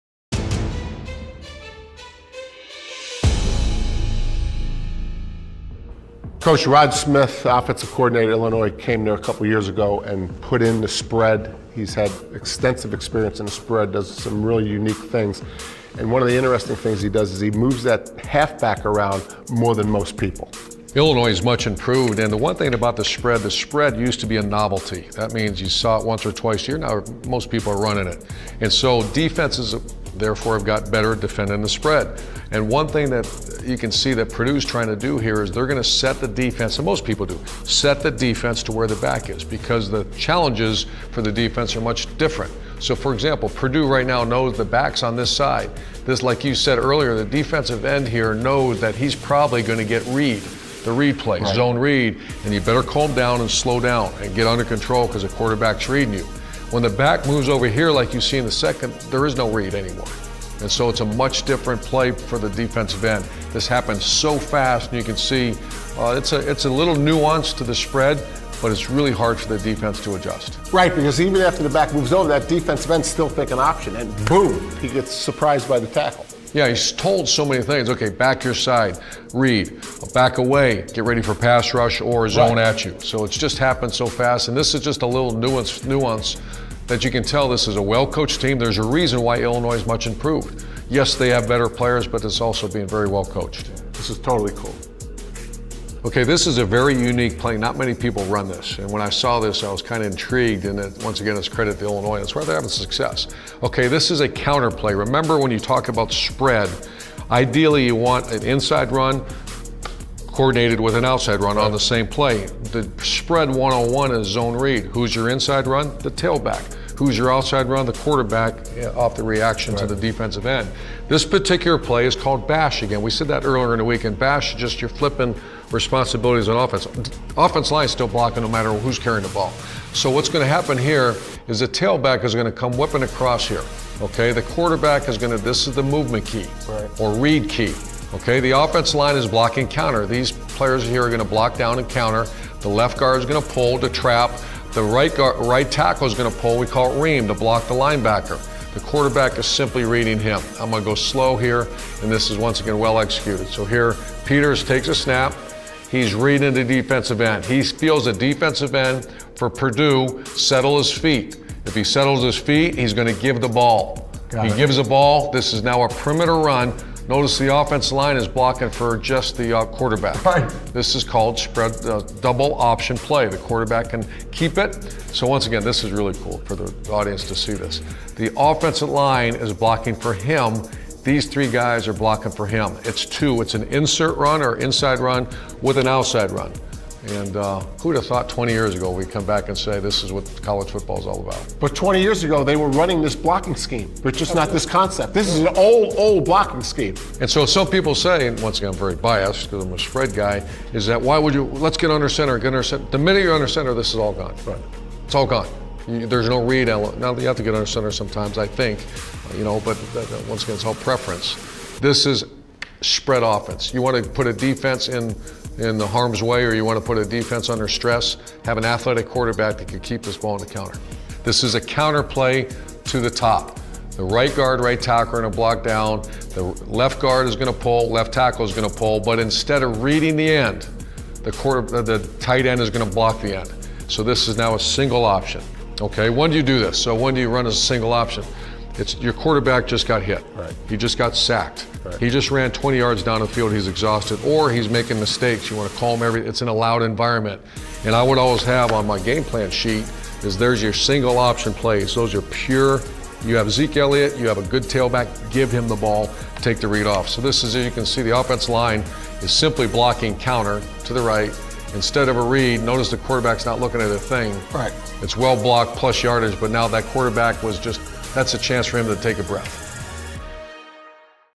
Urban Analysis : Breaking Down Illinois' Spread Offense | B1G Football
Illinois' offensive production has been much-improved the last two years under Rod Smtih and Urban Meyer explains why.